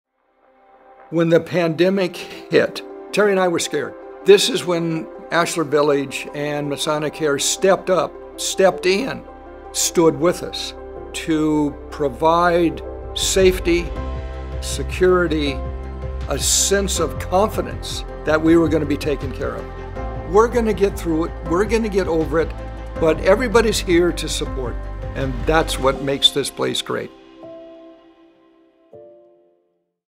This Is Masonicare- Ashlar Village Commerical
Masonicare at Ashlar Village residents talk about how supported they felt by the staff during the COVID-19 pandemic and the peace of mind living there provides.